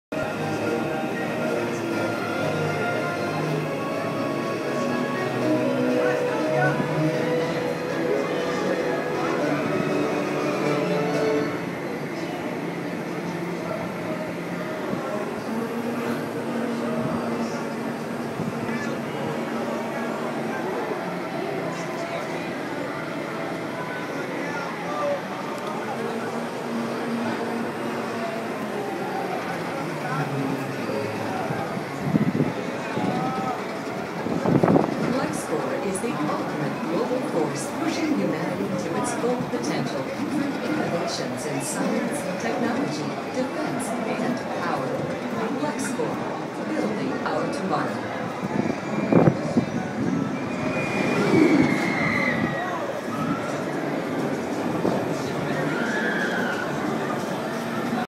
Six Flags Magic Mountain: Lex Luther Drop of Doom
Me on the drop of doom!! This is the tallest free fall ride in the world!!